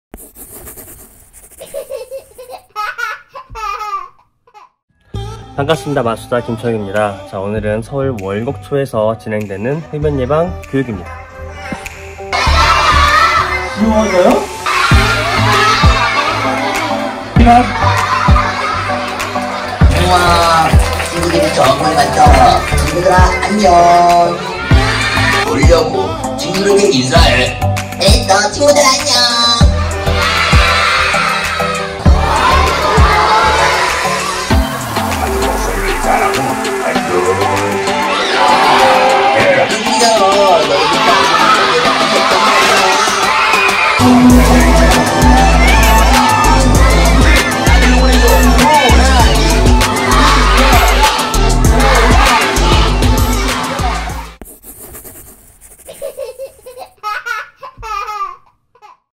서울월곡초등학교 흡연예방교육 [2019-10-23]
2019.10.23 월곡초등학교 에서 김철규마술사의 마술공연과
인형극으로 배우는 흡연예방교육이 있었습니다!
재미와 동시에 교육까지!